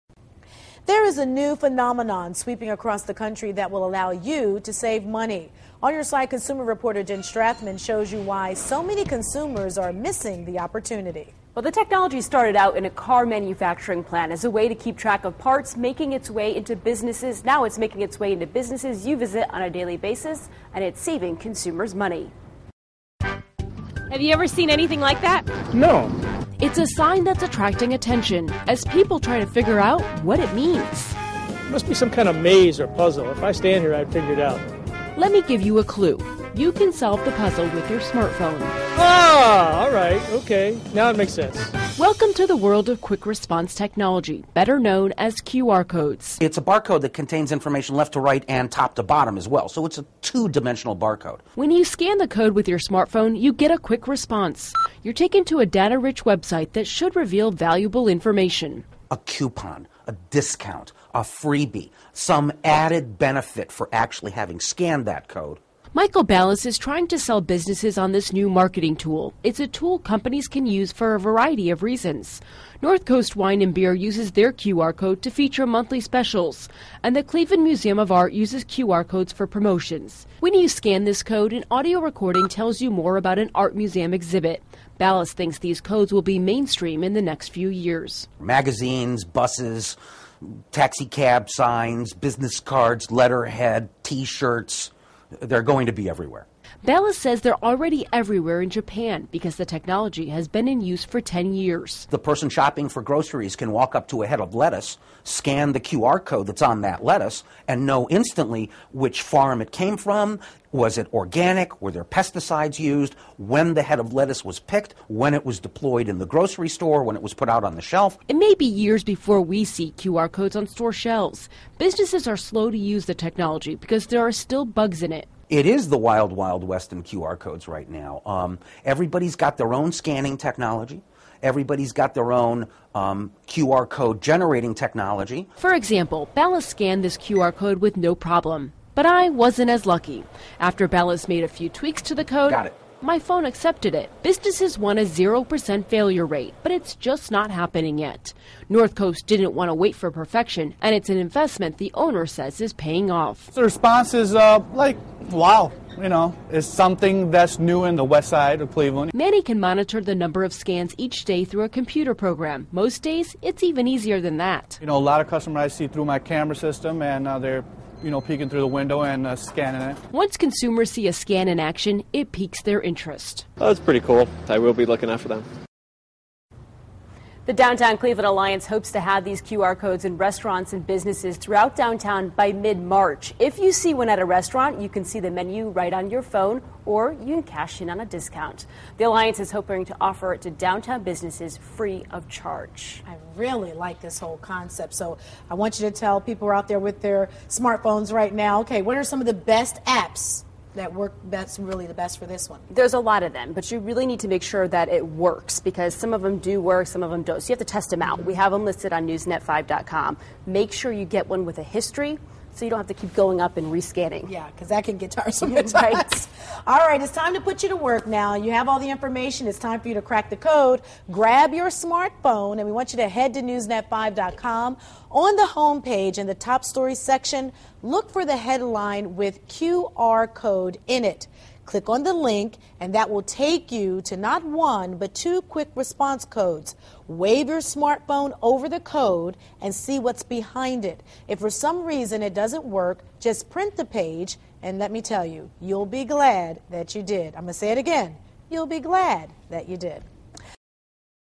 QR Codes
Jenn Strathman reports on quieck response codes.